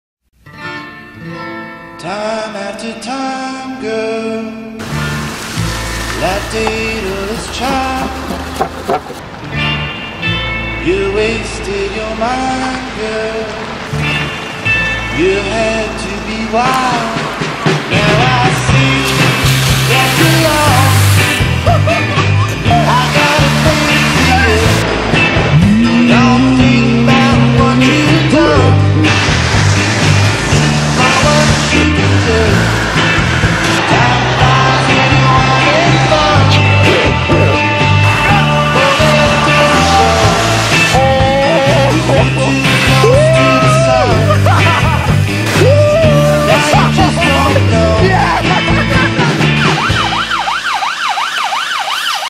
The urban surfing
Him as they drive by while he stands on a surfboard, simulating a true surfing experience!